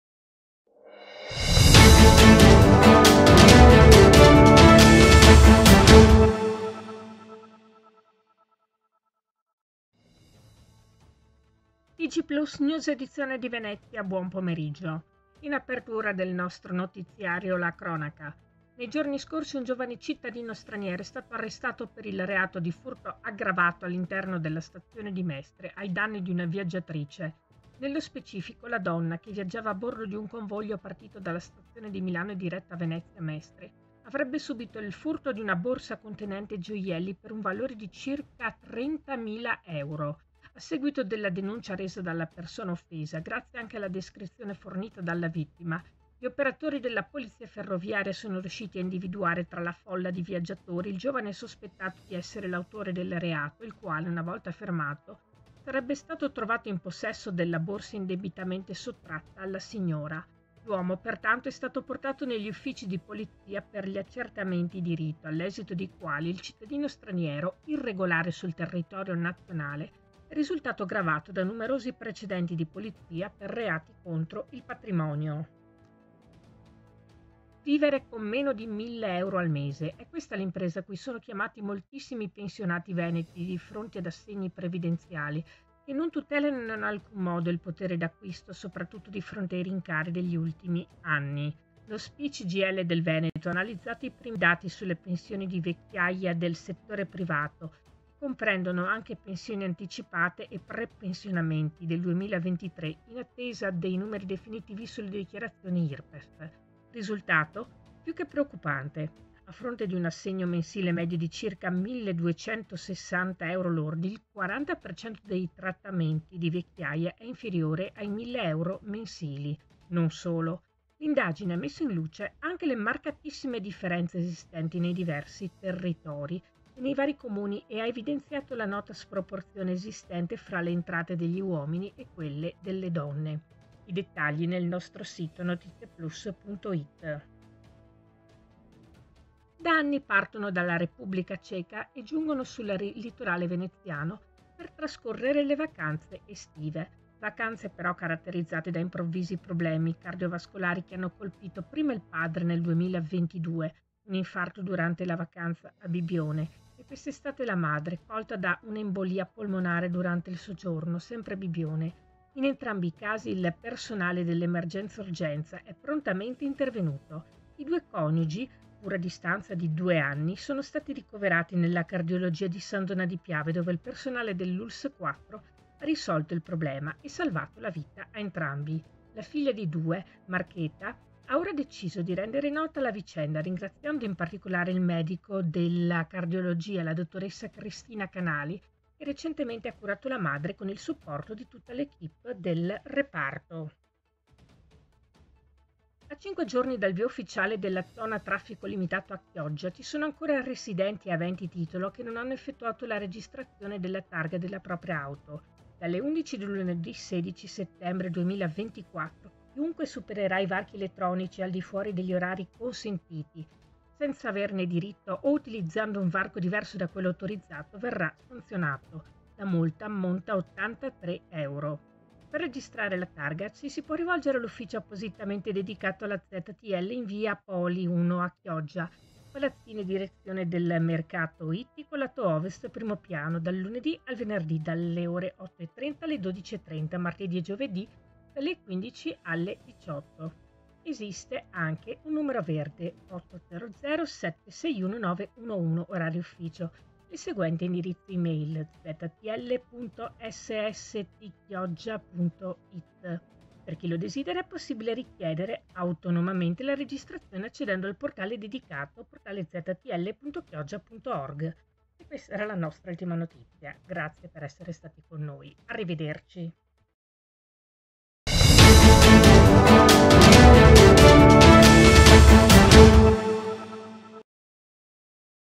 Ruba una borsa con 30mila euro di gioielli: arrestato - TG Plus NEWS Venezia
Nei giorni scorsi, un giovane cittadino straniero è stato arrestato per il reato di furto aggravato all’interno della Stazione di Mestre, ai danni di una viaggiatrice. Nello specifico la donna, che viaggiava a bordo di un convoglio partito dalla stazione di Milano e diretto a Venezia Mestre, avrebbe subito il furto di una borsa contenente gioielli per un valore di circa 30.000 euro. A seguito della denuncia resa dalla persona offesa, grazie anche alla descrizione fornita dalla vittima, gli operatori della Polizia Ferroviaria sono riusciti ad individuare tra la folla di viaggatori il giovane sospettato di essere l’autore del reato il quale, una volta fermato, sarebbe stato trovato in possesso della borsa indebitamente sottratta alla signora. L'uomo, pertanto, è stato portato negli Uffici di Polizia per gli accertamenti di rito all’esito dei quali il cittadino straniero, irregolare sul territorio nazionale, è risultato gravato da numerosi precedenti di polizia per reati contro il patrimonio.

Vivere con meno di mille euro al mese. È questa l’impresa a cui sono chiamati moltissimi pensionati veneti di fronte ad assegni previdenziali che non tutelano in alcun modo il potere d’acquisto, soprattutto di fronte ai rincari degli ultimi anni. Lo Spi Cgil del Veneto ha analizzato i primi dati sulle pensioni di vecchiaia del settore privato (che comprendono anche pensioni anticipate e prepensionamenti) del 2023 in attesa dei numeri definitivi sulle dichiarazioni Irpef. Risultato? Più che preoccupante. A fronte di un assegno mensile medio di circa 1.260 euro lordi, il 40% dei “trattamenti” di vecchiaia è inferiore ai mille euro mensili. Non solo. L’indagine ha messo in luce anche le marcatissime differenze esistenti nei diversi territori e nei vari comuni e ha evidenziato la nota sproporzione esistente fra le entrate degli uomini e quelle delle donne.  I dettagli nel nostro sito Notizieplus.it.

Da vari anni partono dalla Repubblica Ceca e giungono sul litorale veneziano per trascorrere le vacanze estive. Vacanze però caratterizzate da improvvisi problemi cardiovascolari che hanno colpito prima il padre nel 2022, un infarto durante la vacanza a Bibione, e questa estate la madre, colta da una embolia polmonare durante il soggiorno sempre a Bibione. In entrambi i casi il personale dell’emergenza-urgenza è prontamente intervenuto. I due coniugi, pur a distanza di due anni, sono stati ricoverati nella Cardiologia di San Donà di Piave dove il personale dell’Ulss 4 ha risolto il problema e salvato la vita ad entrambi. La figlia dei due, Markéta, ha ora deciso di rendere nota la vicenda ringraziando in particolare il medico della Cardiologia, la dottoressa Cristina Canali che recentemente ha curato la madre con il supporto di tutta l’equipe di reparto.

A 5 giorni dal via ufficiale della Zona a traffico limitato a Chioggia, ci sono ancora residenti e aventi titolo che non hanno effettuato la registrazione della targa della propria auto. Dalle 11.00 di lunedì 16 settembre 2024 chiunque supererà i varchi elettronici al di fuori degli orari consentiti senza averne diritto o utilizzando un varco diverso da quello autorizzato verrà sanzionato. La multa ammonta a 83,00 euro.  Per registrare la targa ci si può rivolgere all’Ufficio appositamente dedicato alla ZTL in via G. Poli, 1 - Chioggia (Palazzine Direzione del Mercato Ittico, lato ovest, primo piano) dal Lunedì al venerdì dalle 8.30 alle 12.30 Martedì e Giovedì dalle 15.00 alle 18.00. Esiste anche un numero verde: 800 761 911 (orari ufficio) e il seguente indirizzo email:  . Per chi lo desidera, è possibile richiedere autonomamente la registrazione accedendo al portale dedicato: portaleztl.chioggia.org.